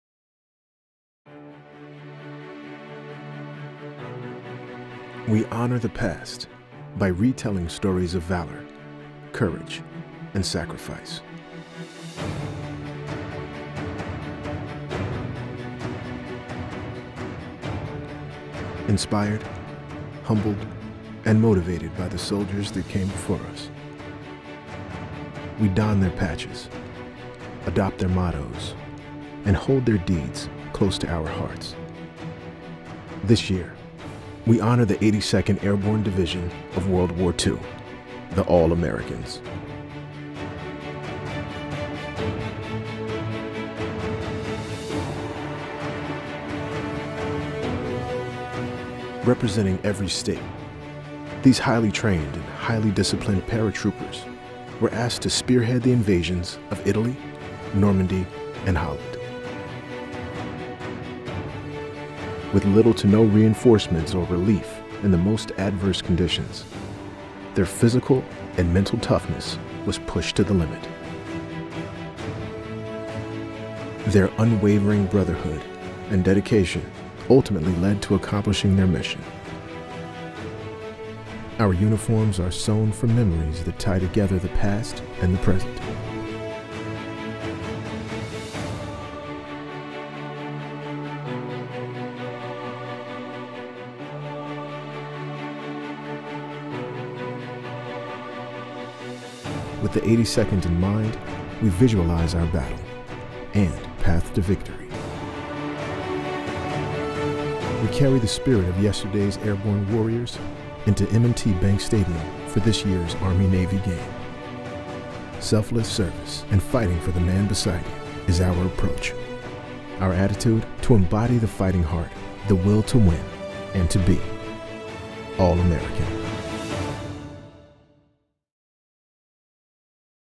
2016 Army-Navy Game Uniform - 82nd Airborne
Army, Nike team up for 2016 Army-Navy Game uniform honoring the 82nd Airborne Division.

Army West Point Football and official apparel sponsor Nike unveil the 2016 Army-Navy Game uniform. This uniform was inspired by the World War II paratroopers of the 82nd Airborne Division. We honor the past by re-telling stories of valor, courage, and sacrifice. Inspired, humbled, and motivated by the soldiers that came before us, we don their patches, adopt their mottos, and hold their deeds close to our hearts.

Learn more about the uniform at BEATNAVYUniform.com